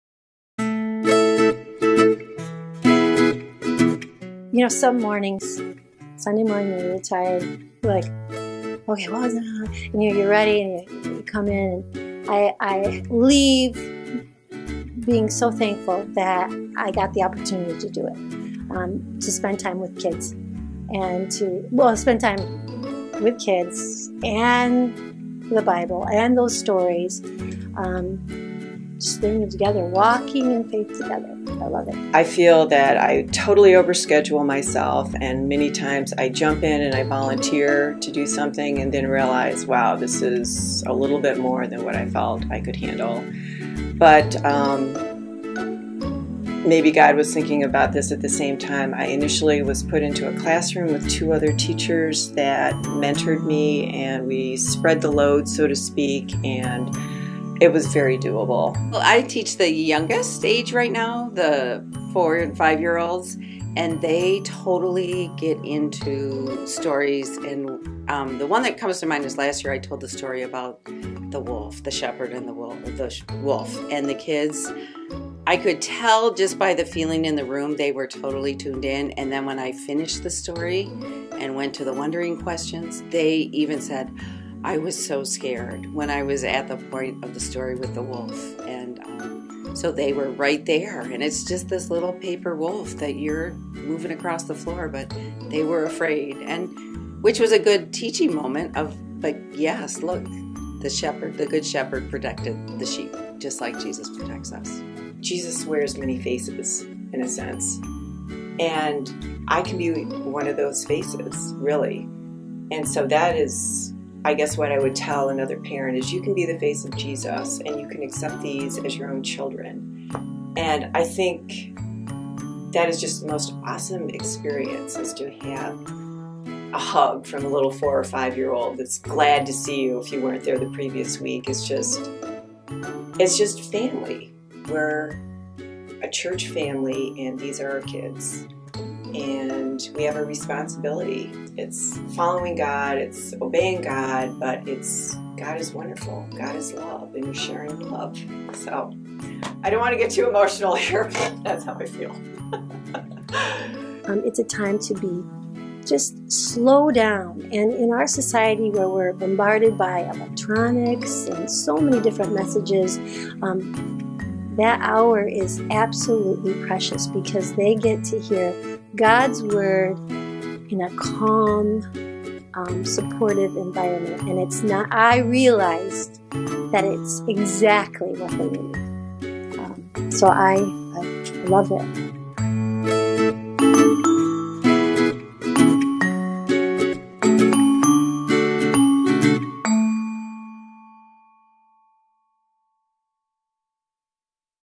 Worship Center Teacher Stories
St. Luke's teachers share their God sightings from connecting with kids on Sunday mornings.